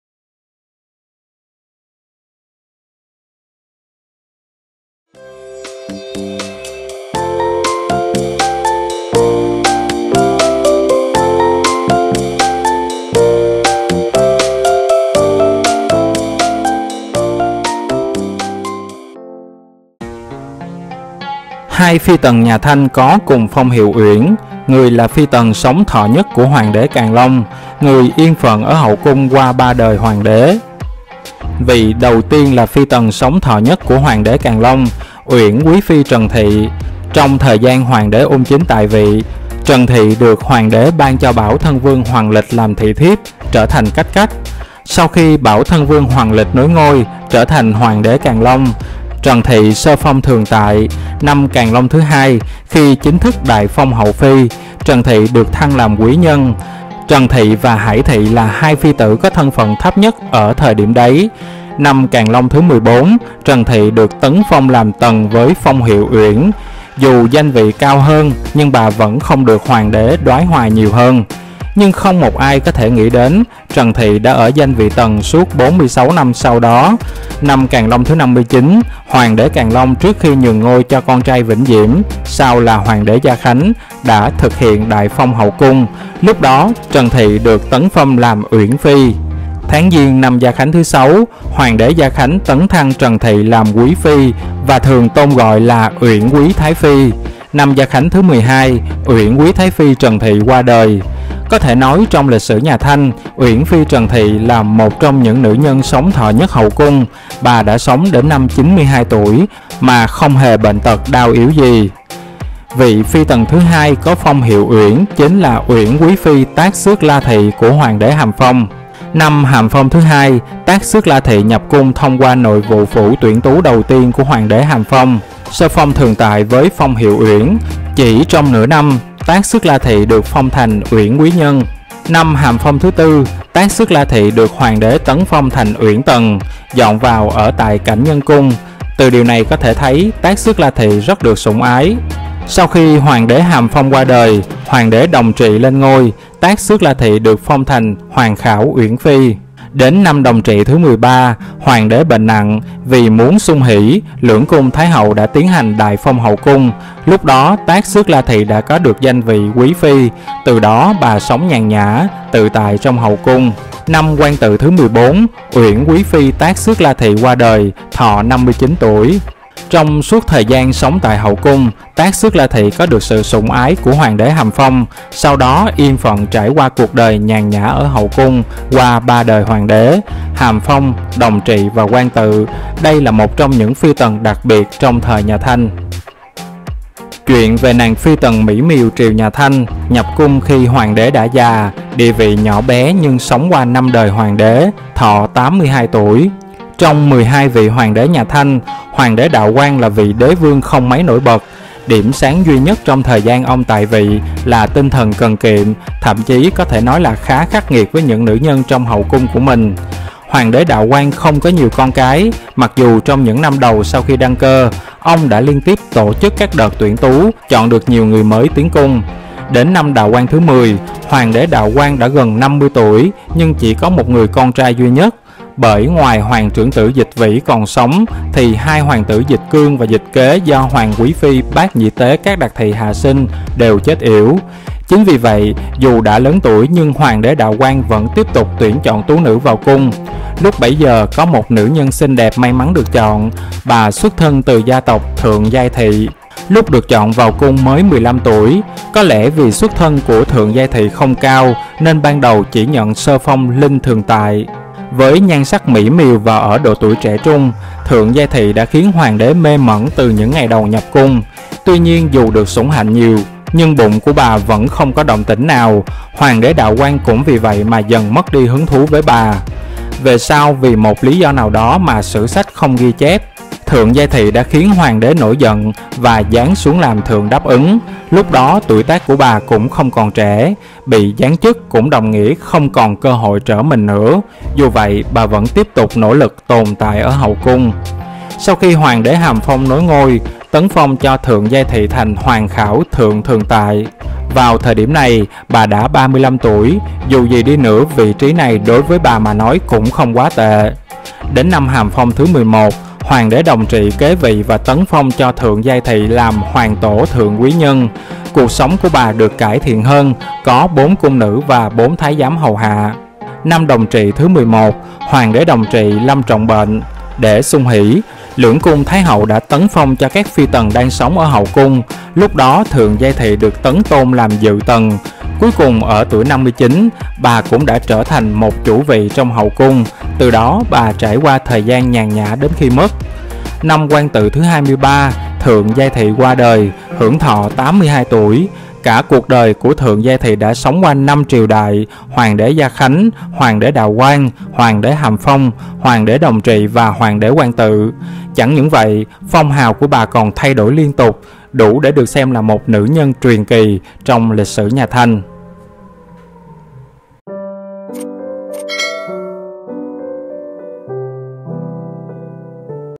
2 PHI TẦN NHÀ THANH có cùng phong hiệu "UYỂN"
2 phi tần nhà Thanh có cùng phong hiệu "Uyển": Người là phi tần sống thọ nhất của Hoàng đế Càn Long, người yên phận ở hậu cung qua 3 đời Hoàng đế

Thượng Giai Thị của Đạo Quang Đế - Nhập cung khi Hoàng đế đã già, địa vị nhỏ bé nhưng sống qua 5 đời Hoàng đế, thọ 82 tuổi